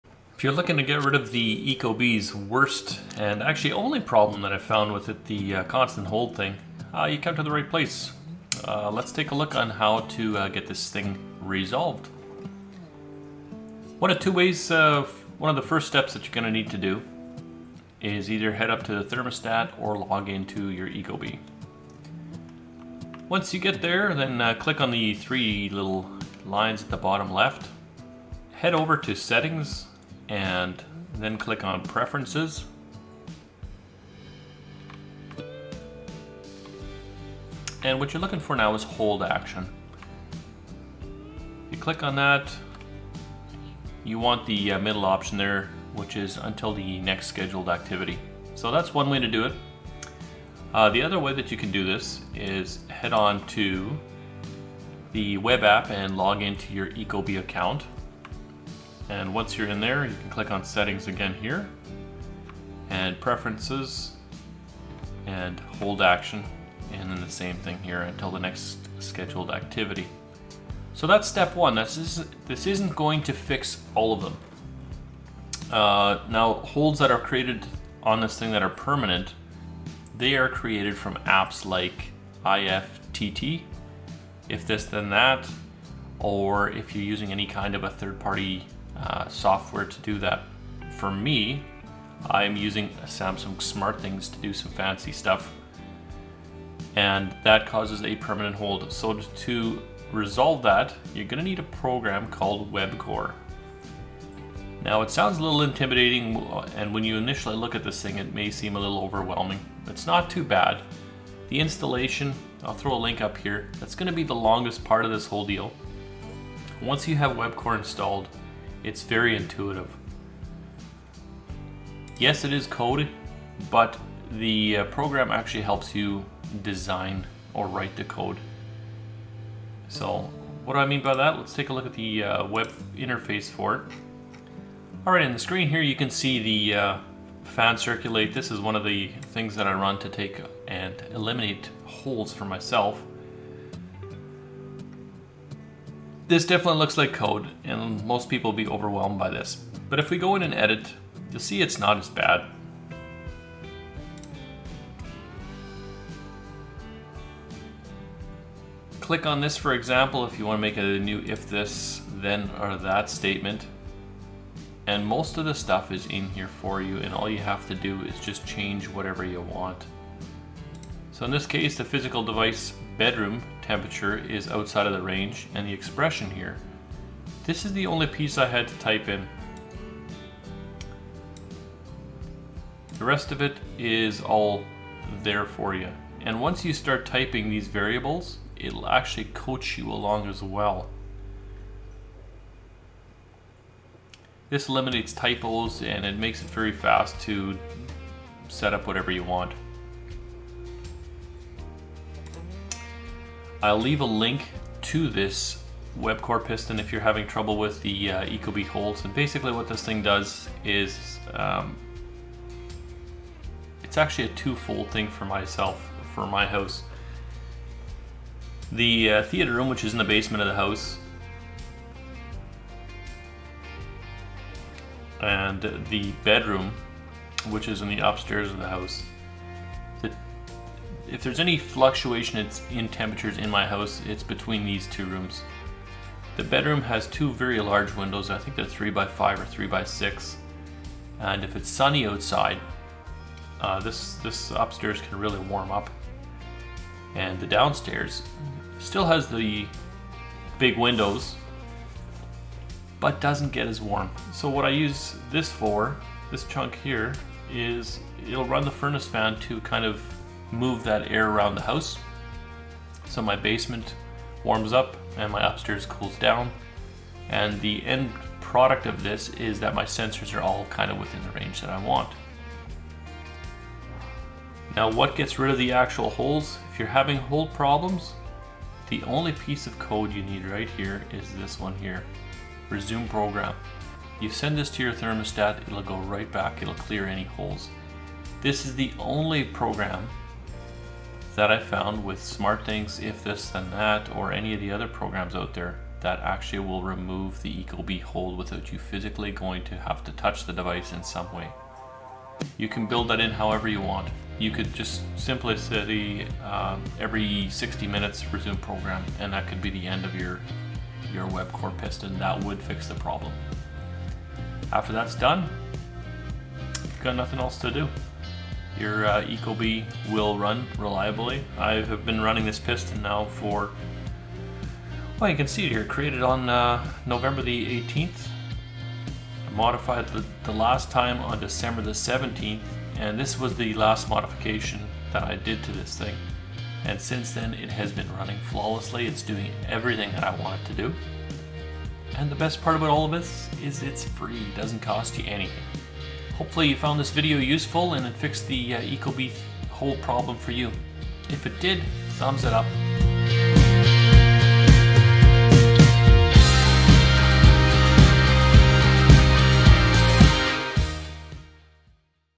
Fix Ecobee Smart Thermostat Holds
I go through the 2 things you'll need to do in order to stop the holds on the Ecobee Smart Thermostat.

***Webcore is no longer working to remove holds, new video coming out December 17, 2021, I've found another working solution.***

Ecobee Thermostat
Ecobee Smart Sensor
Smart Sensor Replacement Battery

Chapters
0:00 Intro
0:25 Thermostat Holds
0:57 Web App Holds
1:15 Causes
1:43 Webcore
2:35 Webcore Code
4:36 Webcore Anti Hold Code